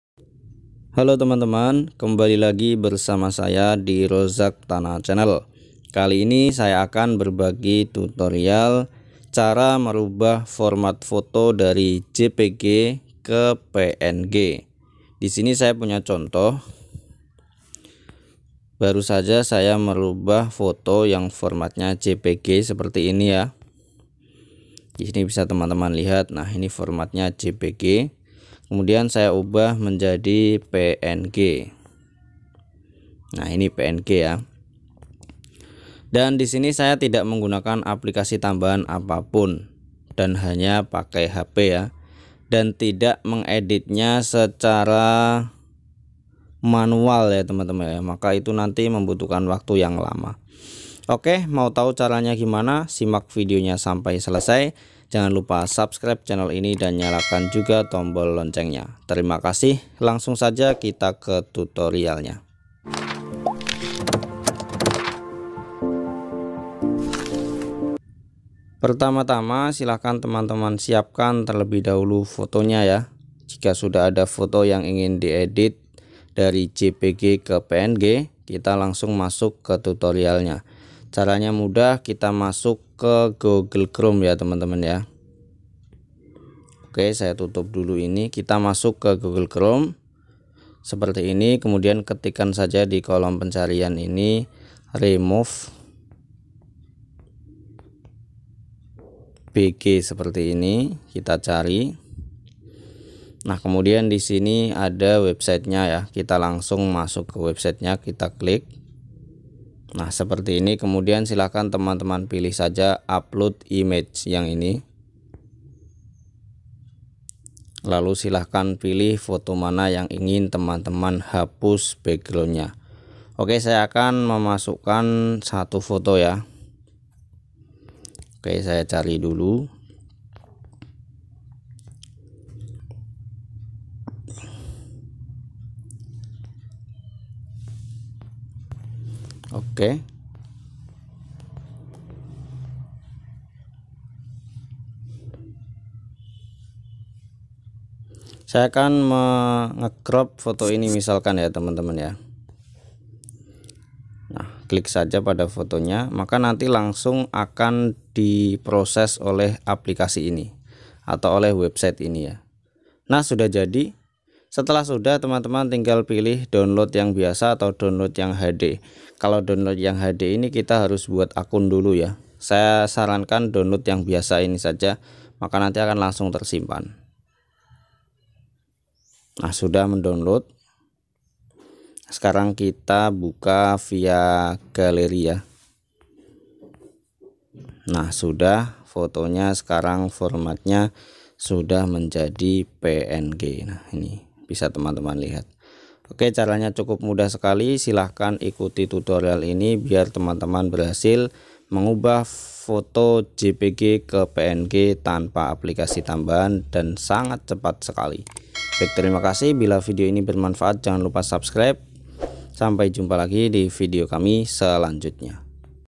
Cara Merubah Foto JPG Ke PNG Di Hp Android Tanpa Aplikasi - Mudah & Cepat
Berikut ini adalah tutorial mengubah foto JPG ke PNG secara otomatis di hp android. Cara ini juga tidak membutuhkan aplikasi tambahan.

Teman-teman bisa menghapus background secara otomatis dengan mudah dan cepat sekali. Untuk itu simak videonya sampai selesai agar teman-teman berhasil merubah format foto dari JPG ke PNG tanpa harus mengedit secara manual.

Demikian, semiga video ini bermanfaat.